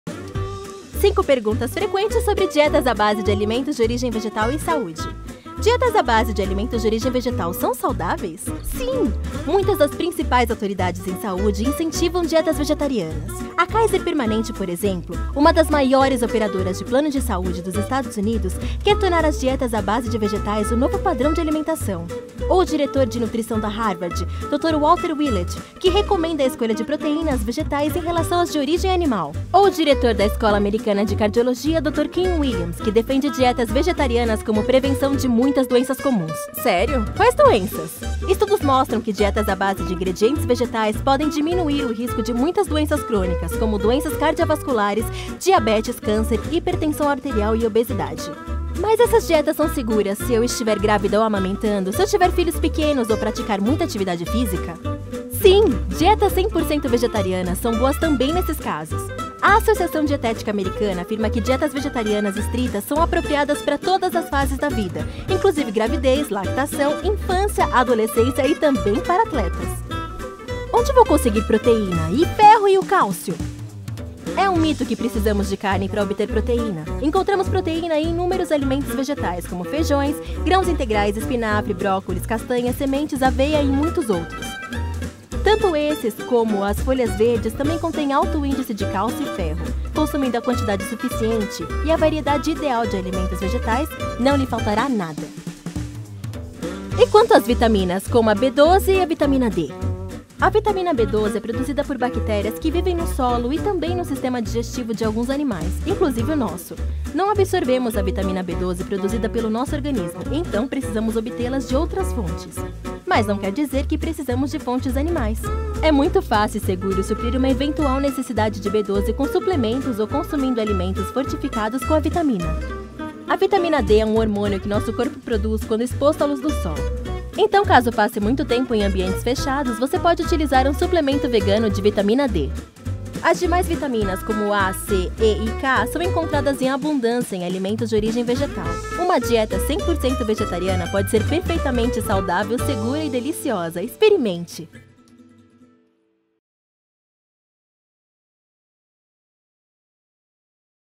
Dúvidas frequentes sobre vegetarianismo
Tem dúvidas sobre vegetarianismo? Alimentos ricos em ferro, cálcio? Como obter vitamina B12?

Aqui explicamos tudo pra você de forma simples e fácil!